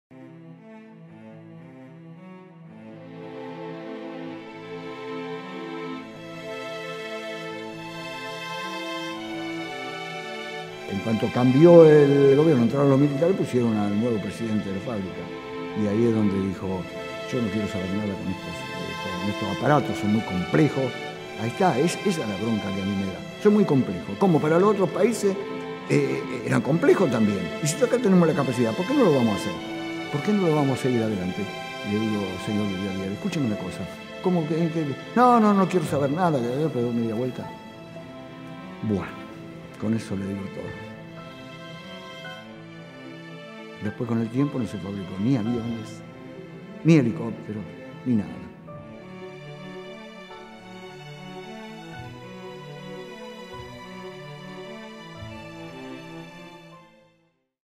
Retrato de lo que no debe volver a suceder II
Augusto Cicaré, un argentino que lucha para que Argentina pueda volar en su propio cielo.